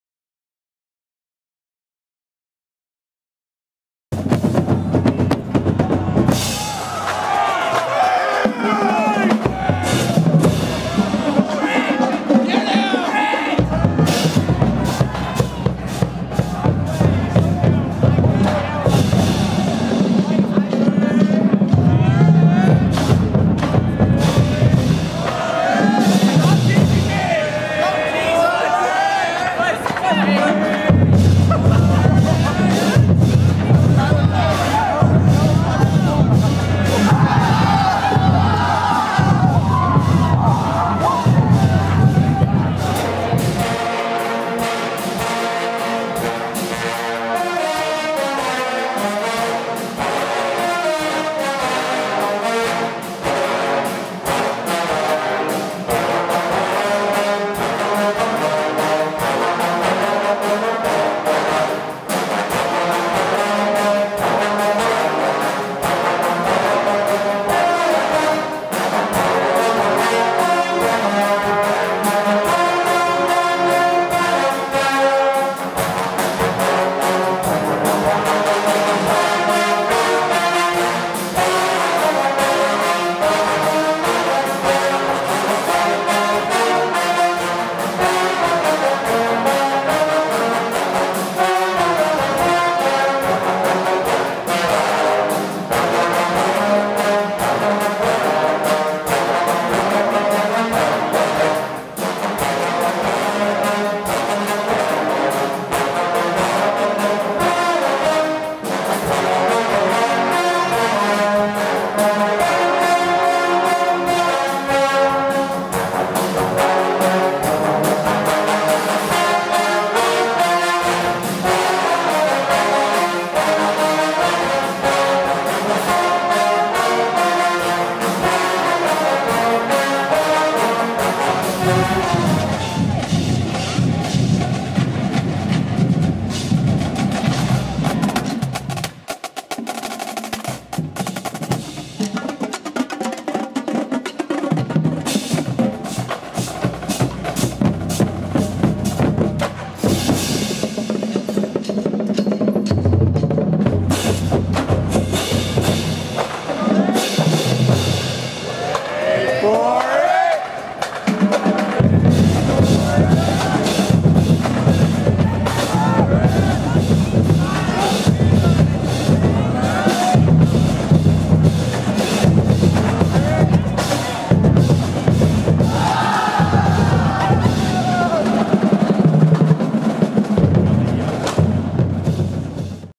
Georgia Tech Marching Band - Trombones 2011
This is the Georgia Tech Trombone section as seen on Nov 10 & 26, 2011 on the GT campus. On Oct 28th they appeared at the GT Ferst Center Surround Sound concert.